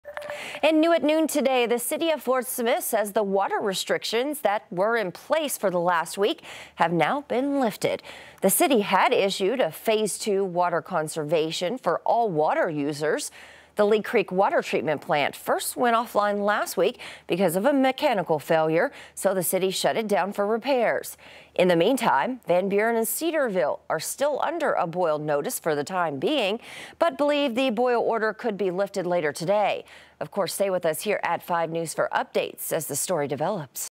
Fort Smith water conservation lifted
The water conservation issued on Aug. 4 has now been lifted.